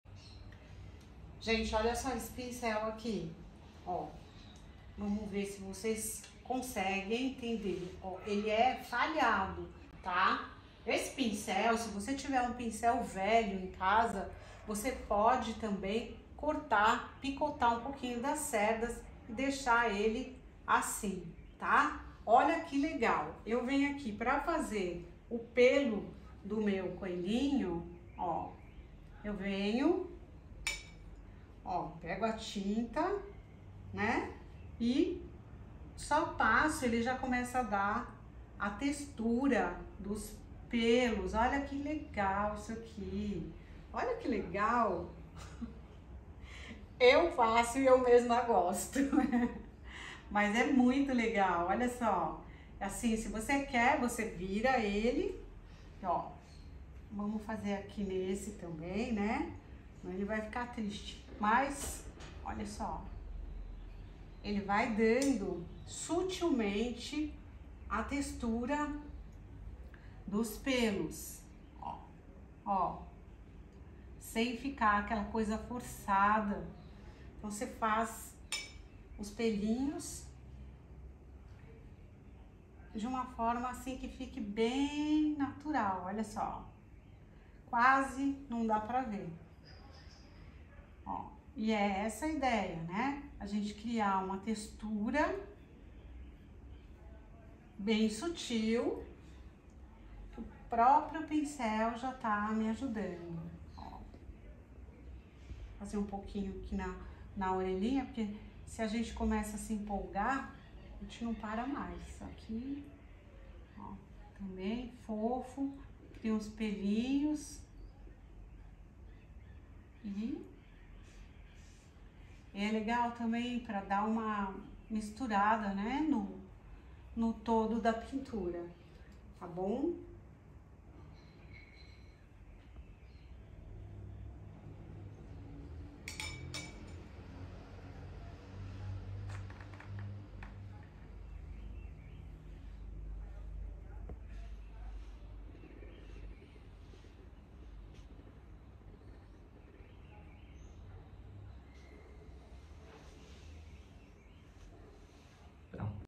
Como fazer pelinhos na Arte em Parede
Aprenda a fazer pelinhos na Arte em Parede usando essa técnica simples.